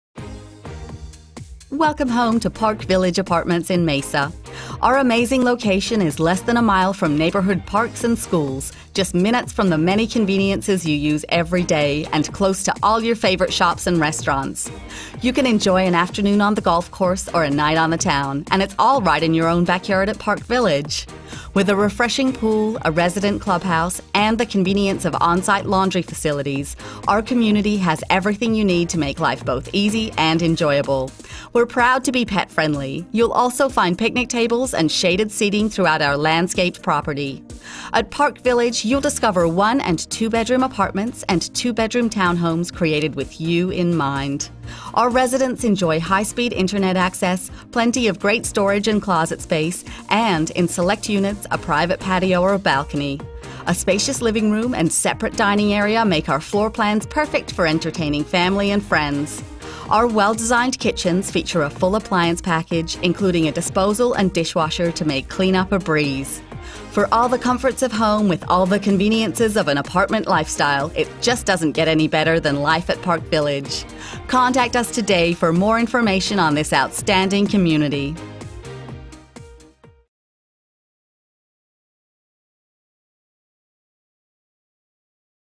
Park Village Apartments in Mesa, AZ - ForRent.com
Park Village Apartments for rent in Mesa, AZ on ForRent.com: Availability, pricing and special promotions subject to change daily. Park Village Apartments, located in Mesa, Az, offers spacious 1 and 2 bedroom apartments and 2 bedroom townhomes featuring private patios or balconies in select homes, extra storage, ceiling fans, window coverings, hardwood style flooring, high speed internet access, resident clubhouse, refreshing swimming pool, on-site laundry facility, is close to public transportation and accepts credit card payments and housing vouchers. - 226 N Hobson